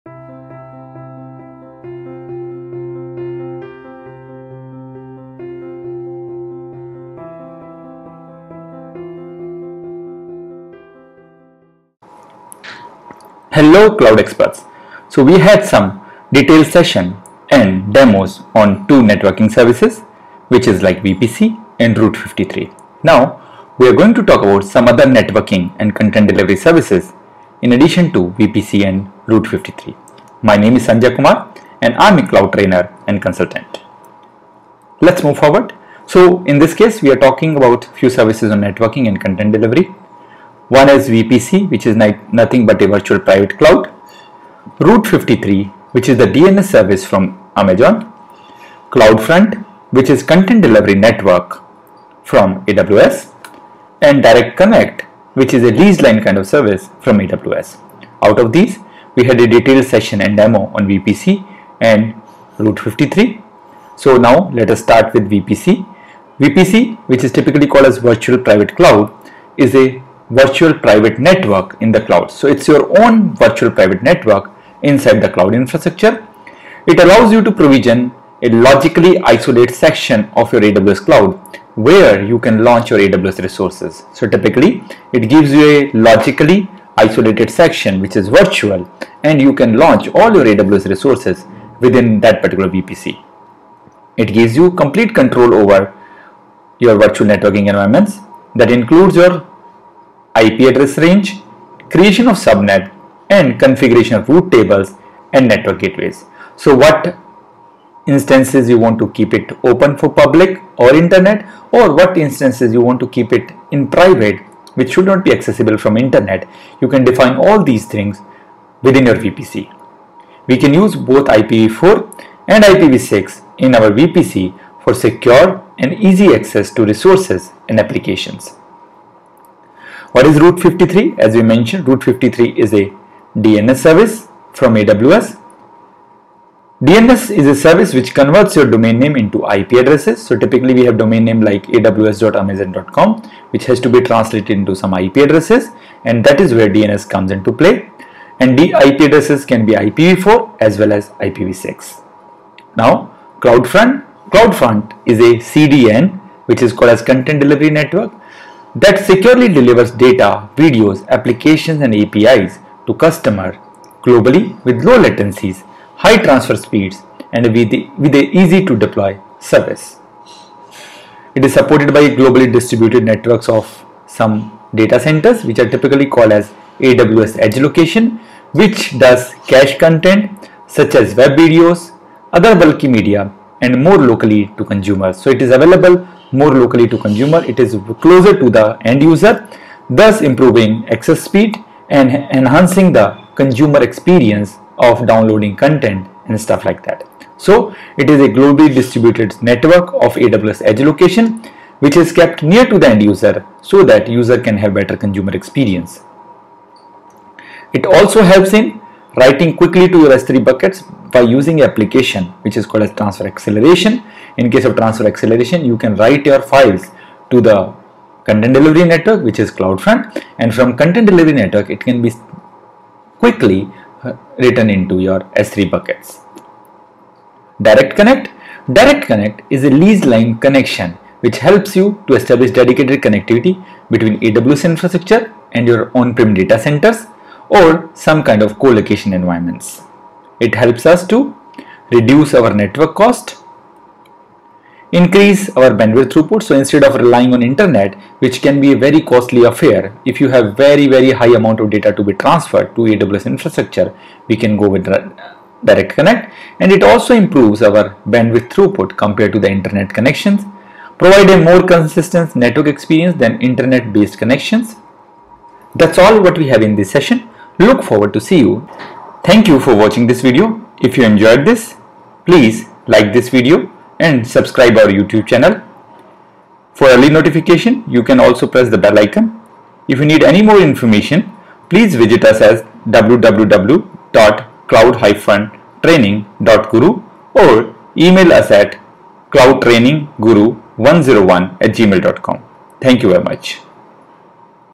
| Networking Services in AWS | VPC | Route53 | CloudFront|
In this session, we will talk about AWS Networking & CDN Services like VPC , Route53 , CloudFront

Networking Services in AWS ,